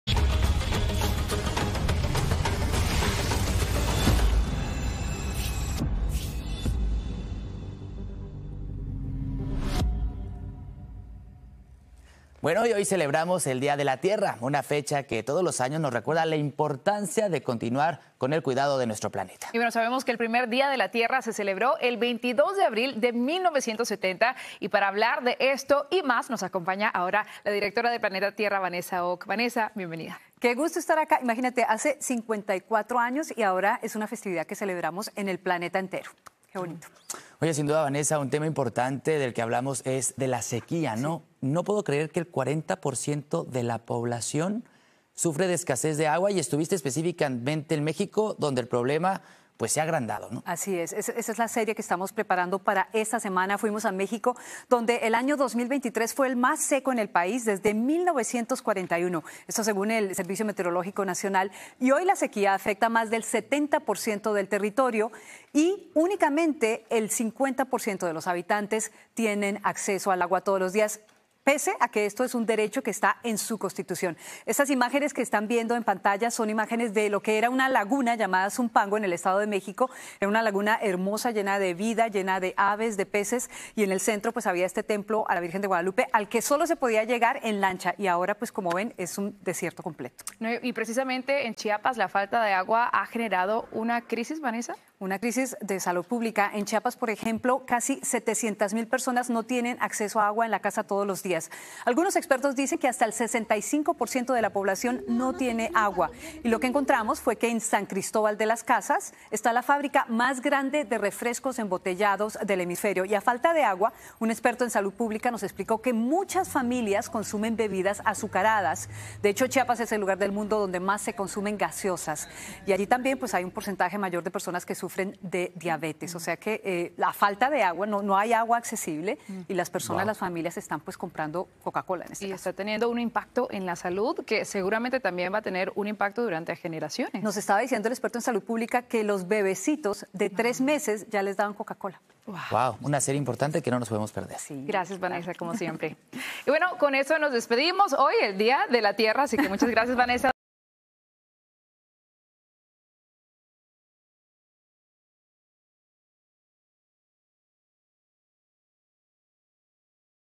Planeta Tierra: Una ruda sequía afecta al 40% de la población mundial | Noticias Telemundo
Video oficial de Noticias Telemundo. El año 2023 fue el año más seco que ha registrado México desde 1941, afectando especialmente a las comunidades más desfavorecidas. La falta de agua destaca en la celebración 54 del Día de la Tierra.